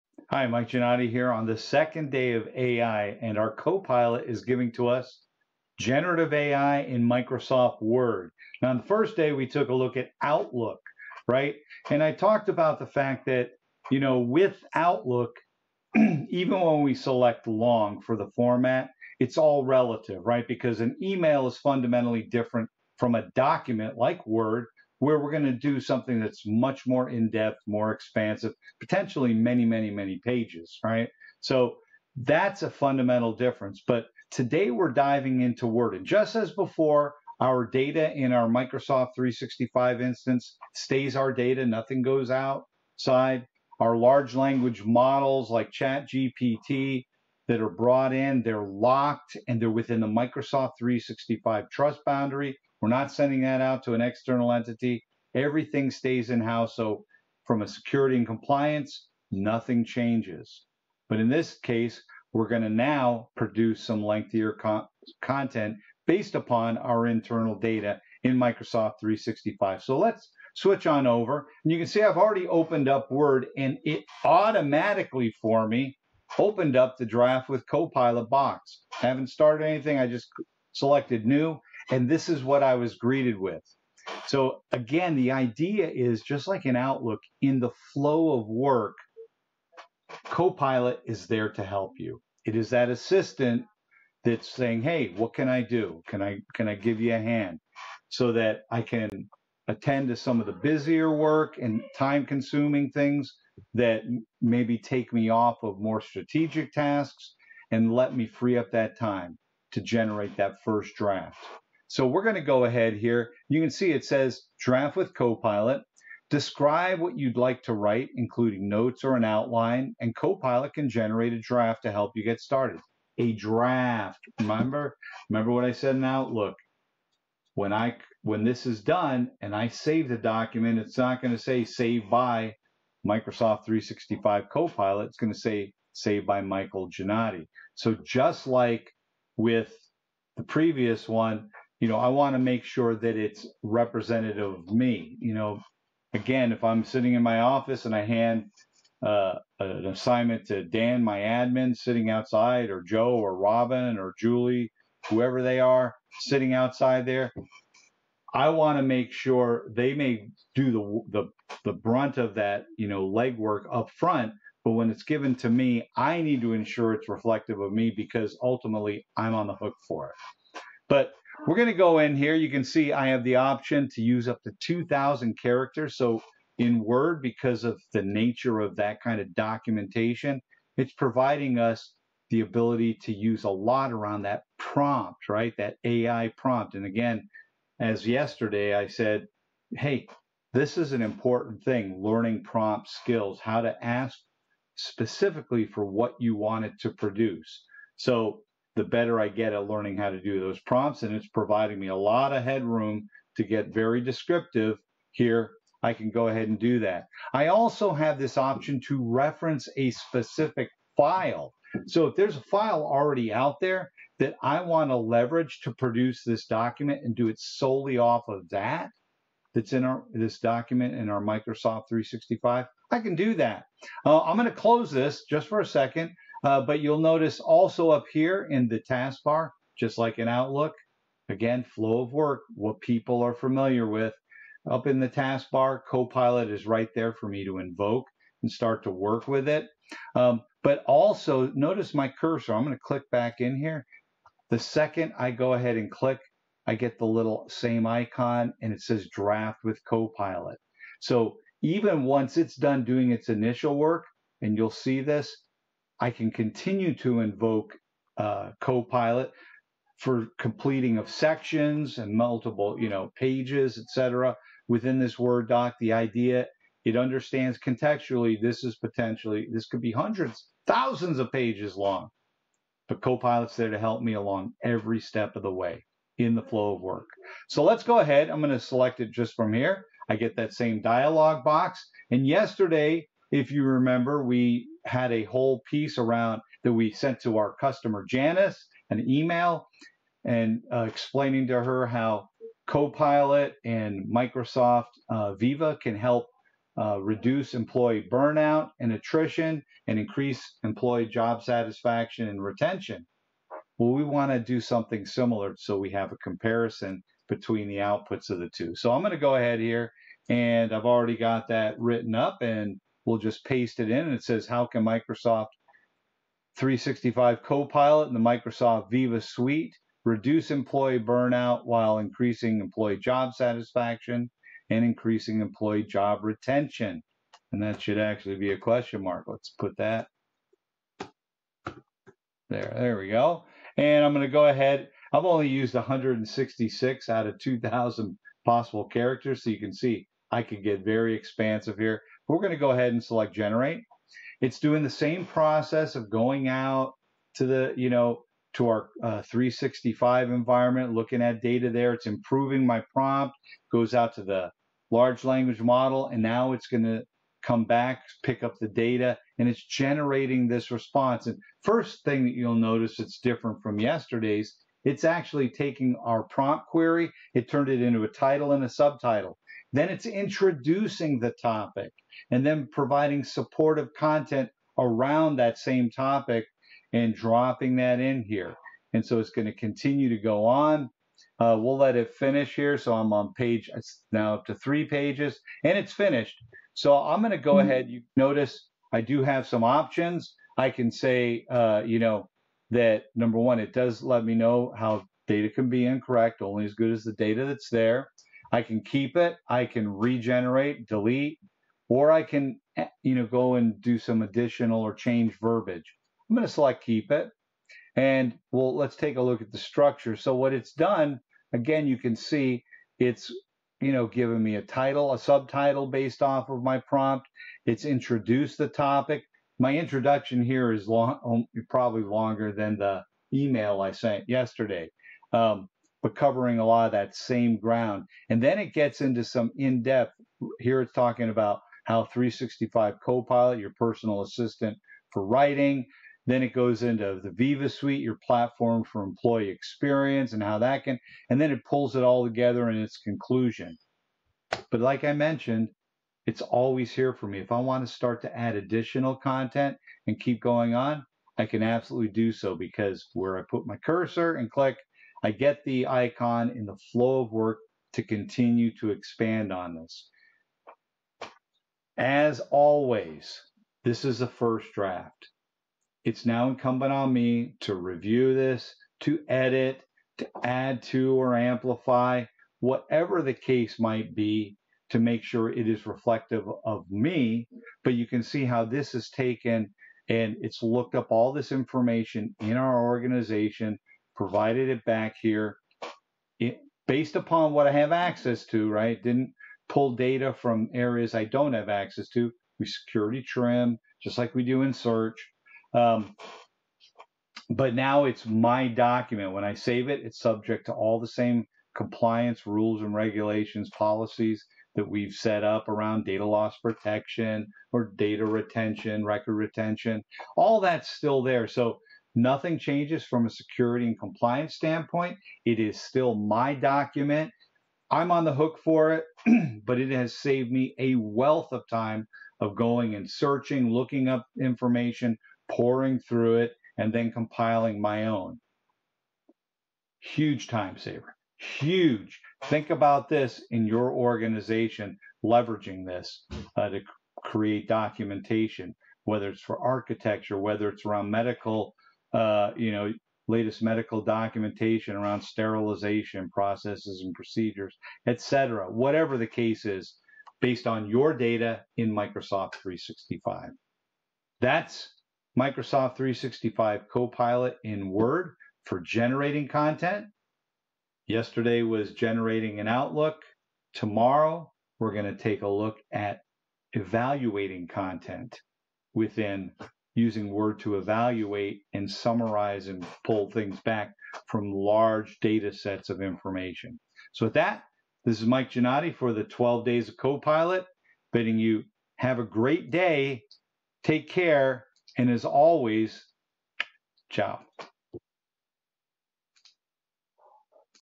The 12 Days of Copilot – Day 2 Microsoft 365 Copilot in Word – HLS Show Me How
Welcome to the 12 Days of Copilot Day 2. Each day our AI is giving us a Microsoft 365 Copilot gift. On day 2 our AI is giving us a great assistant for drafting documents in Microsoft Word.
About Microsoft 365 Copilot use in Word:
Microsoft 365 Copilot in Word can benefit users in drafting documents by providing them with relevant and high-quality content suggestions, based on their topic, style, and tone. Copilot can help users to:
- Create engaging and informative documents with suggestions for headings, introductions, summaries, and more.
- Improve the structure and flow of their documents with suggestions for transitions, bullet points, and formatting.
- Enhance the clarity and accuracy of their documents with suggestions for grammar, punctuation, word choice, and readability.
- Customize their documents with suggestions for different audiences, purposes, and formats.